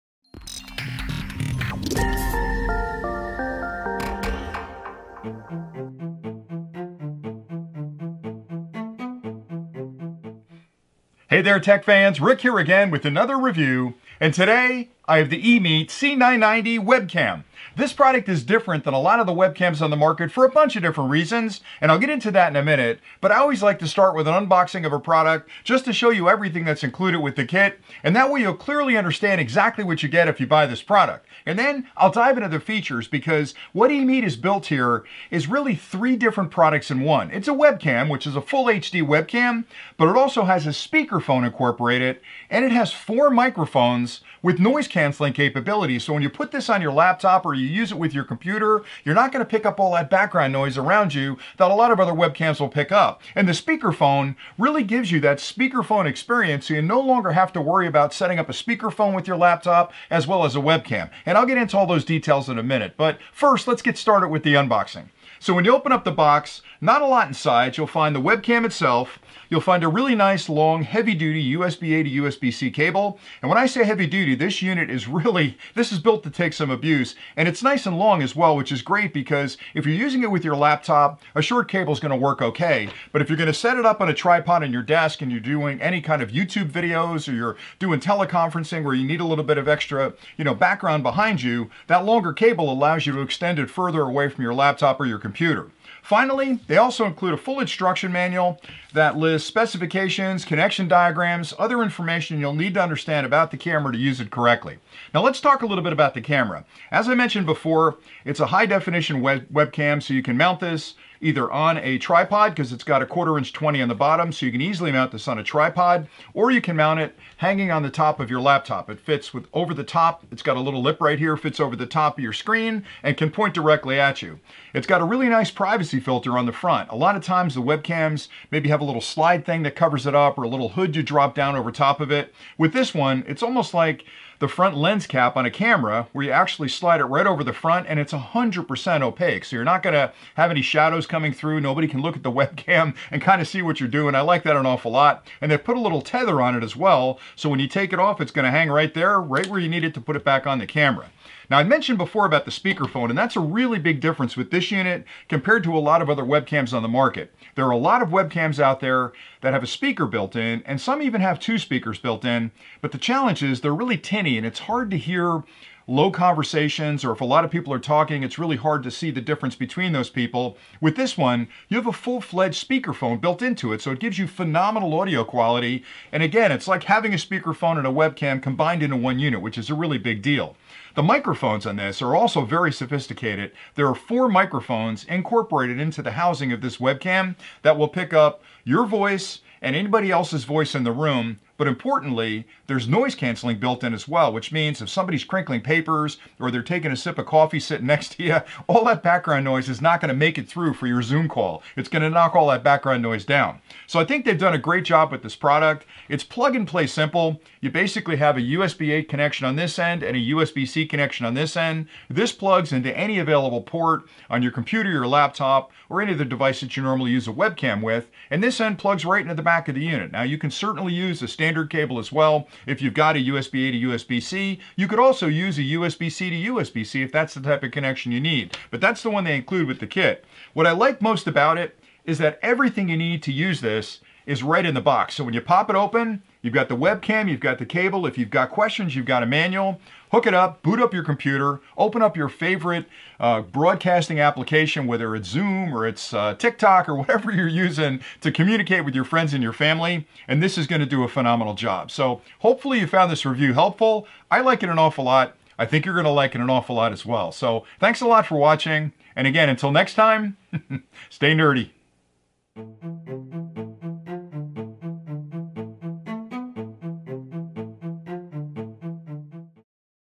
Emeet C990 Webcam - 3 Devices in One - Great if you're working from home!
Emeet C990

If you’re looking for an upgrade to the webcam in your laptop or need one for Zoom calls with friends, the Emeet C990 might be a great choice.  This Emeet C990 webcam is really 3 devices in one – an HD Webcam, 4 Noise-canceling microphones and a speakerphone. With one connection to your computer you can jump on a video call in style. The best part is that it’s reasonably priced and works with all of the major Web Conferencing programs.